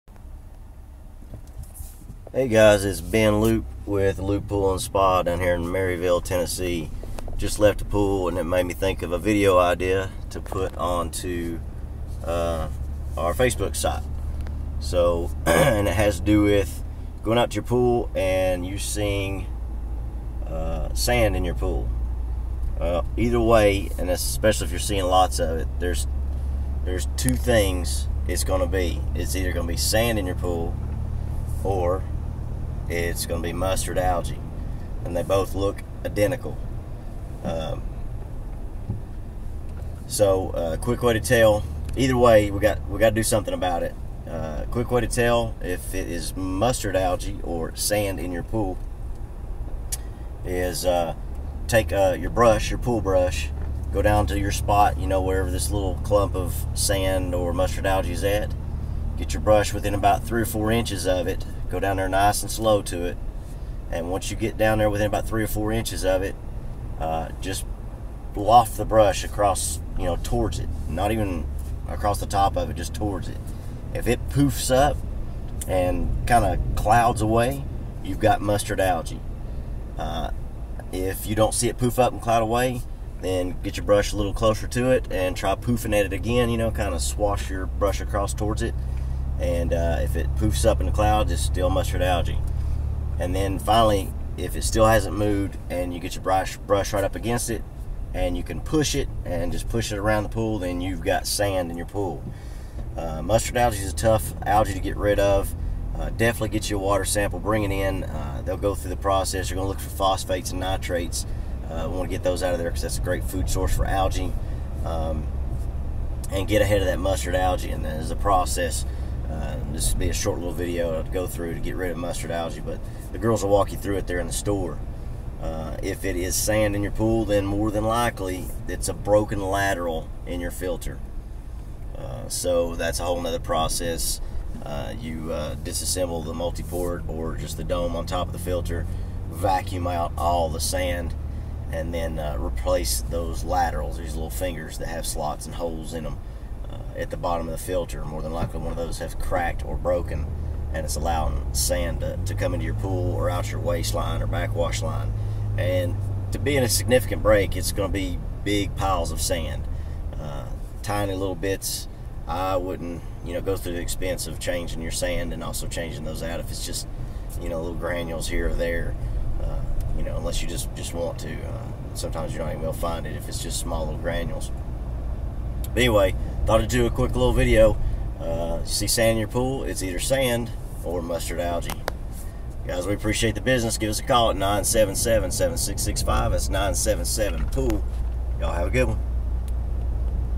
Is it sand or mustard algae?
Ways to quickly tell the difference between sand or mustard algae.